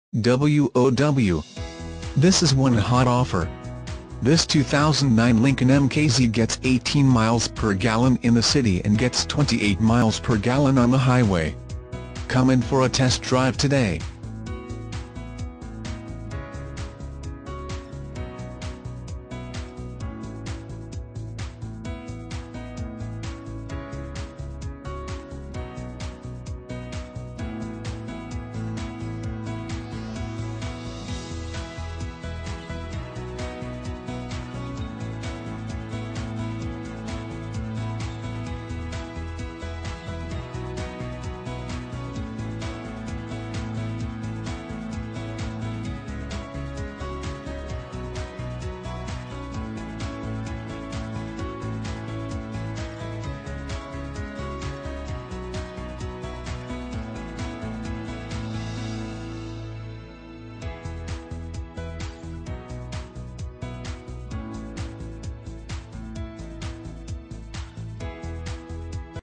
2009 Lincoln MKZ - 4dr Car Milford DE R4620
2009 Lincoln MKZ

Hertrich Ford Lincoln Mercury
1427 Bay Road

WOW! This is one hot offer!  This 2009 LINCOLN MKZ gets 18 miles per gallon in the city and gets 28 miles per gallon on the highway.   Come in for a test drive today!